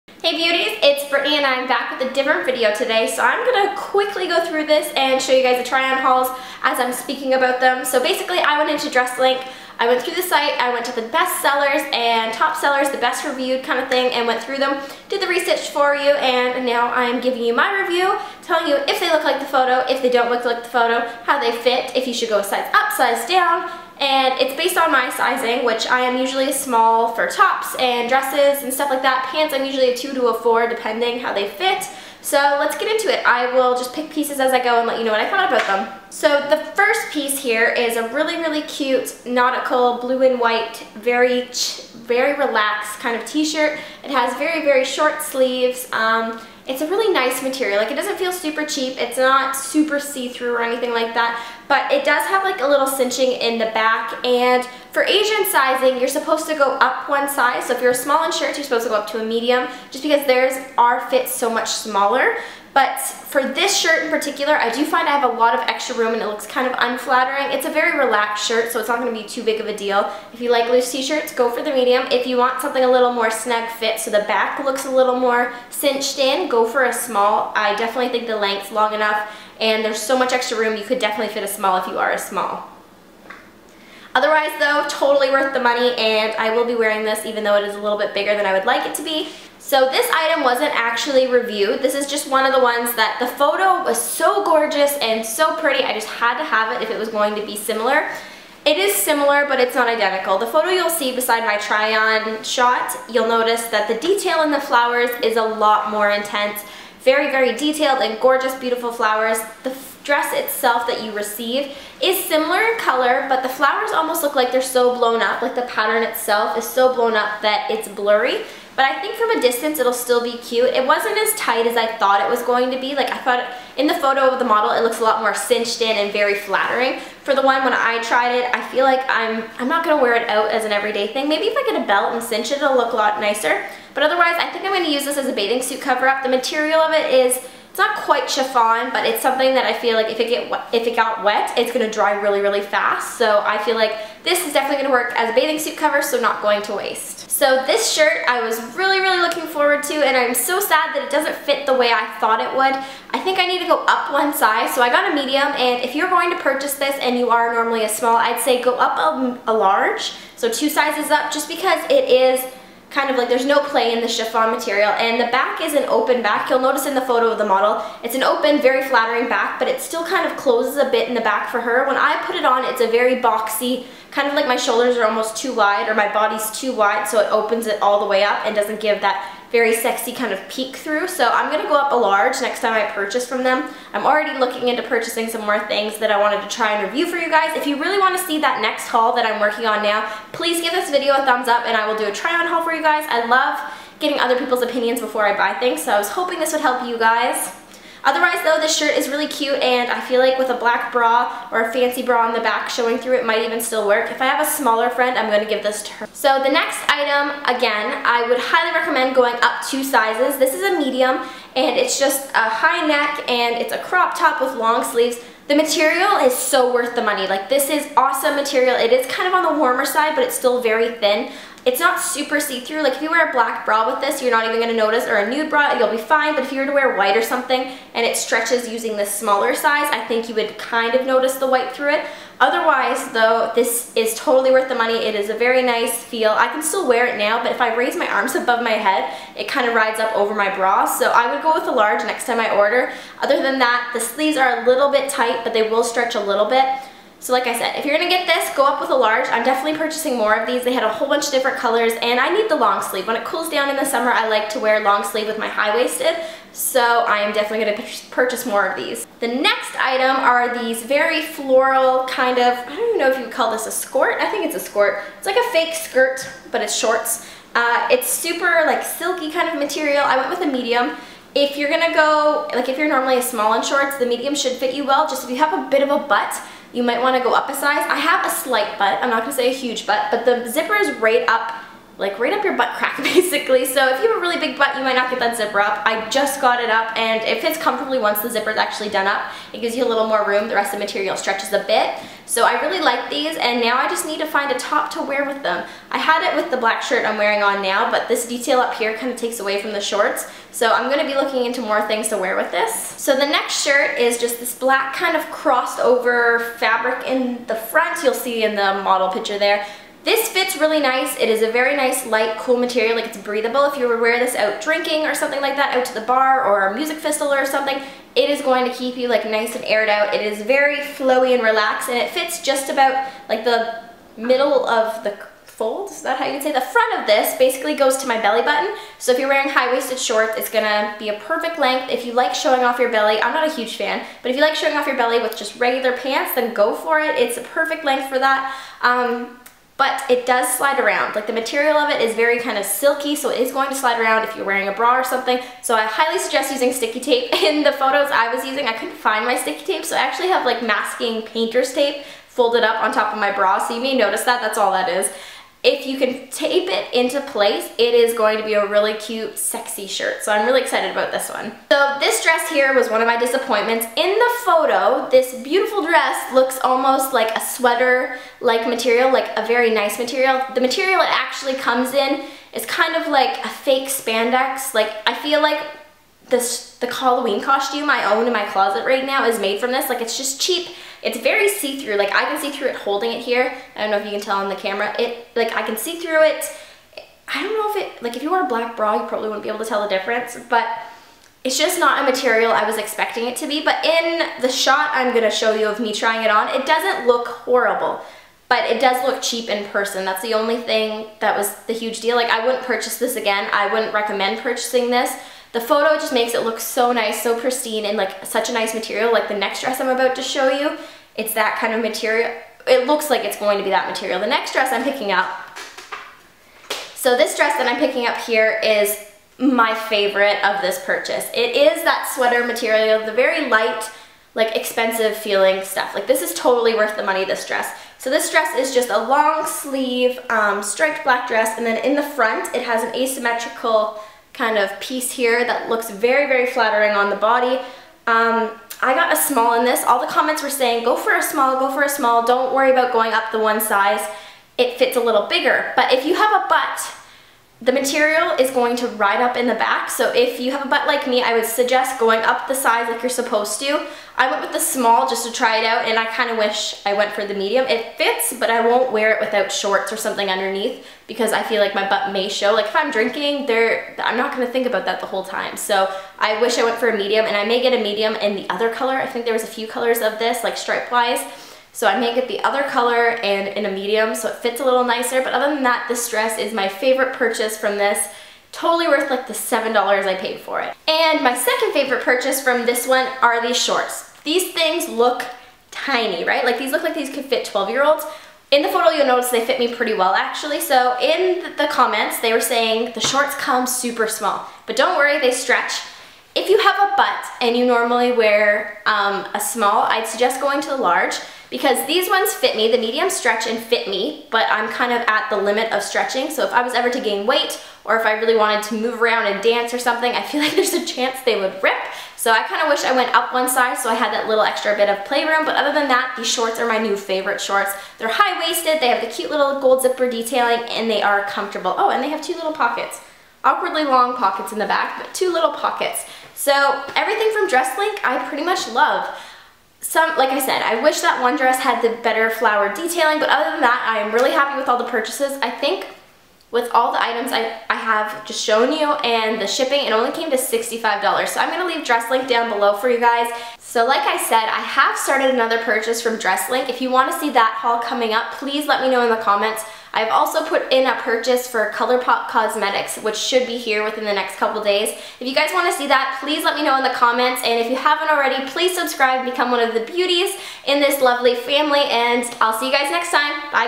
DRESSLINK TRY-ON HAUL - Affordable Summer Clothing | Britany Burnside
I bought, tried on and reviewed some of the BEST SELLERS from dress link to give you the honest truth and all in all it turned out to be totally worth the money!

STAY UPDATED & TALK TO ME!!!

MY FAVOURITE SITES!